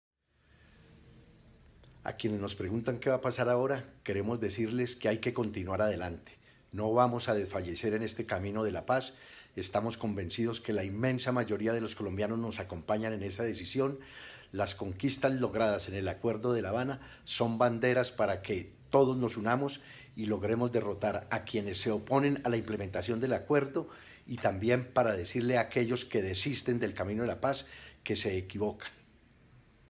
Carlos Lozada se refirió al anunció de Iván Márquez de retomar las armas
Carlos Antonio Lozada, miembro del partido Farc se pronunció sobre el anuncio de Iván Márquez de retomar las armas y conformar un nuevo grupo guerrillero. Lozada dijo que no “van a desfallecer en el camino de la paz” y que “las conquistas logradas con el acuerdo de La Habana son una bandera para derrotar a quienes se oponen a la implementación de los acuerdos”.